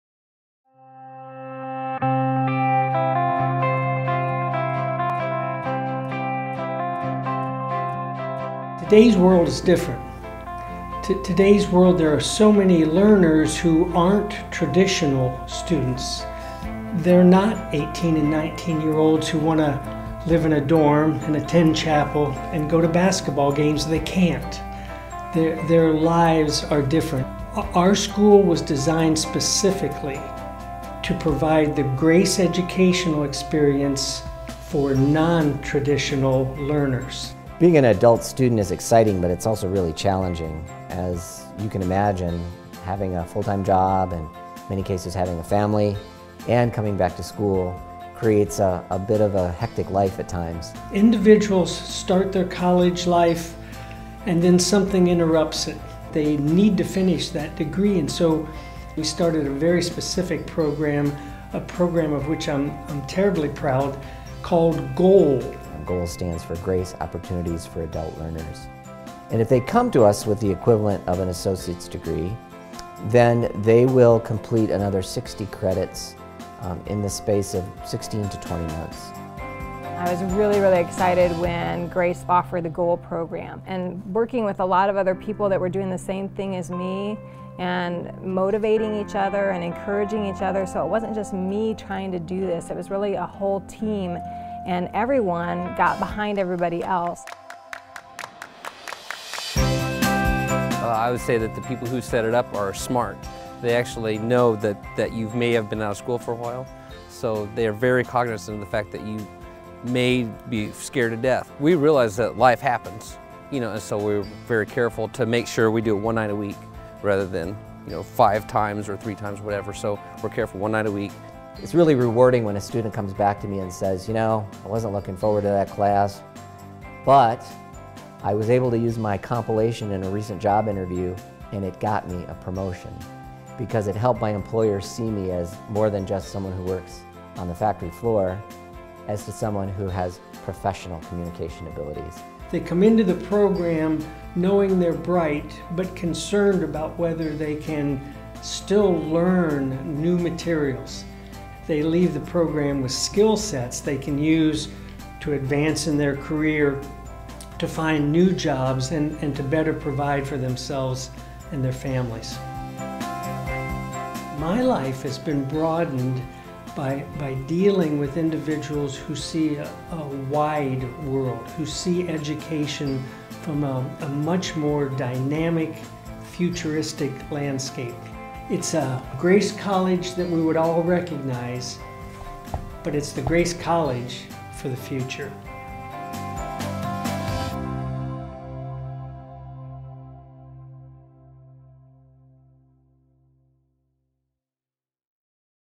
Grace College - School of Professional & Online Education
Balancing family and work, but still want to go back to school and finish your degree? Totally possible at Grace College.